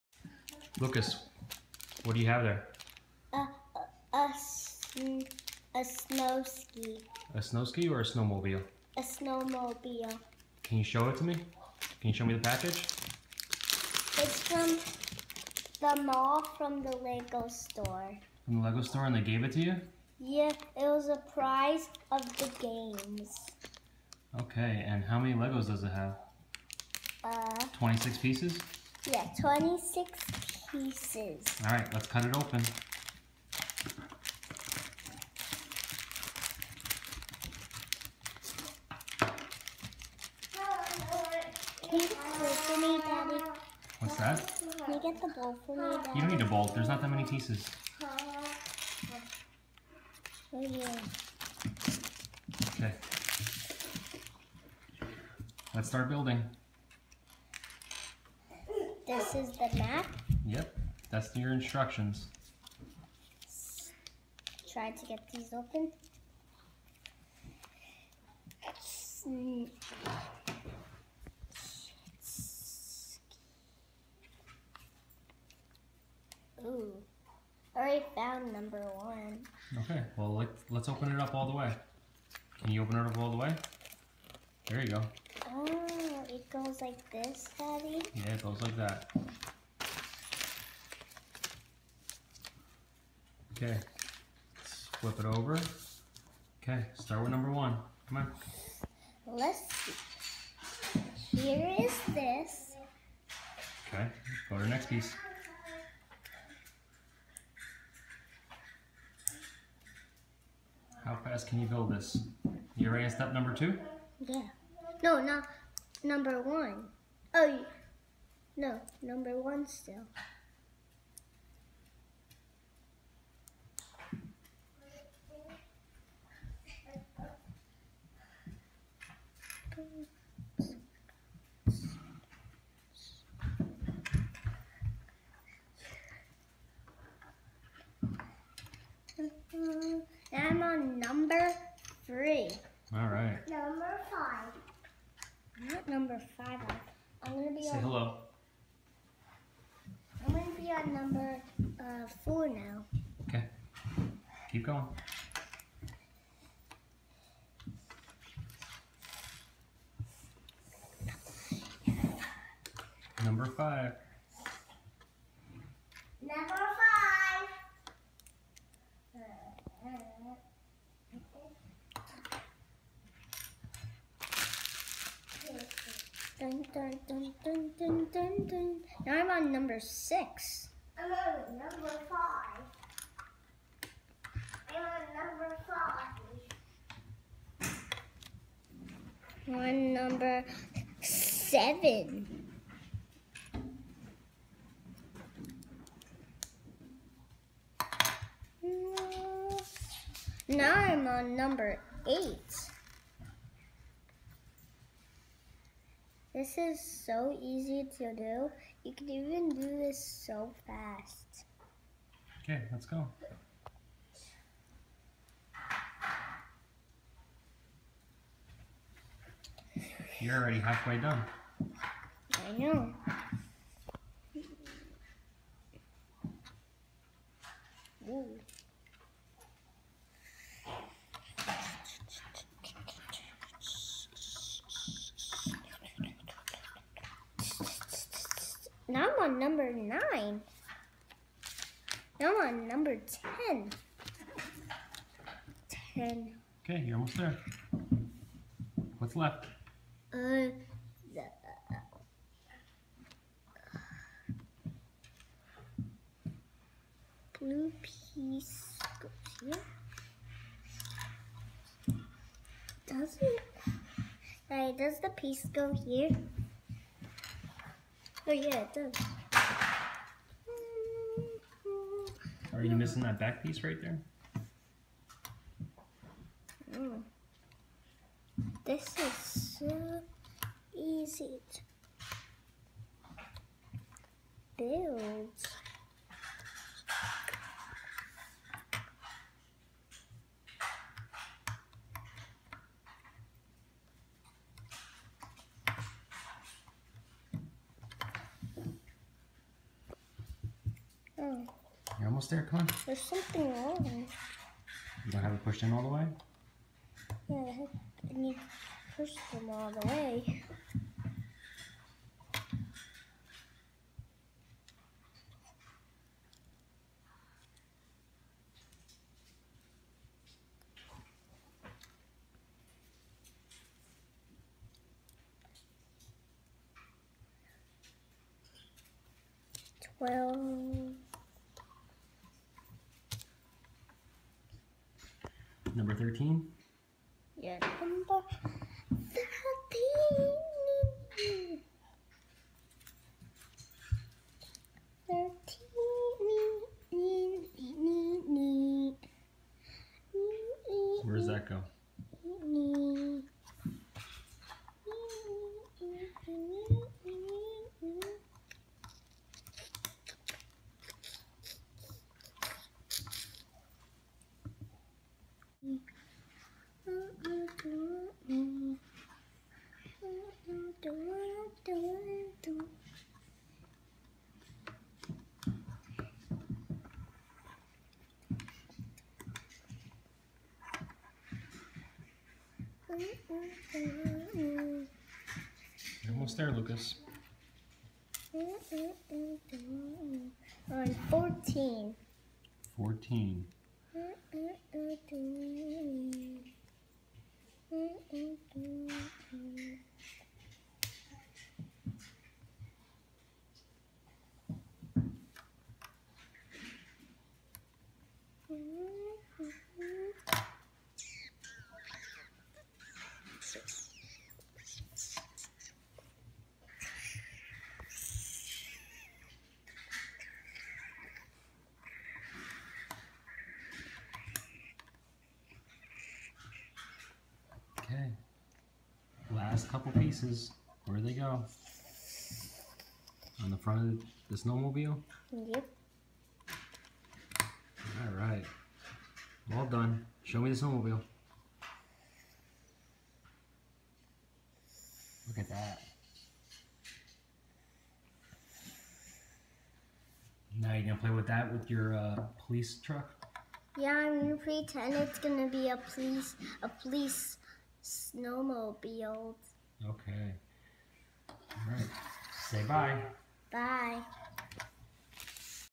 Lucas N Bruno Build Lego Snowmobile set 40209
Lucas received a lego snowmobile from the  Lego store at the mall. He had fun building it.